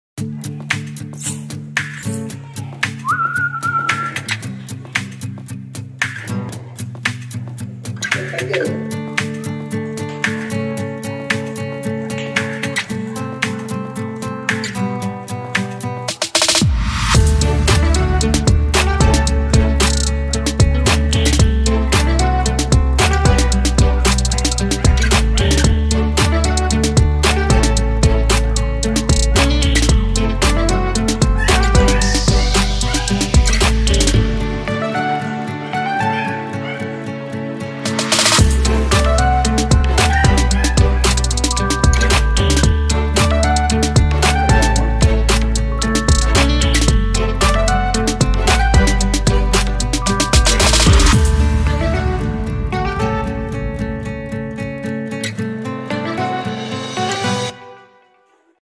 JUSTIN | Chippiparai Dog Feeding |DOGX | Alldogs தமிழ்நாட்டுநாய் வகைகள்
Chippiparai is one of the fastest dogs in the world. They can hit speeds of over 60 kmph, some breeders have recorded 65 kmph as top speed for their dogs
கன்னி நாய்கள்
தமிழ் நாட்டு நாய் வகைகள்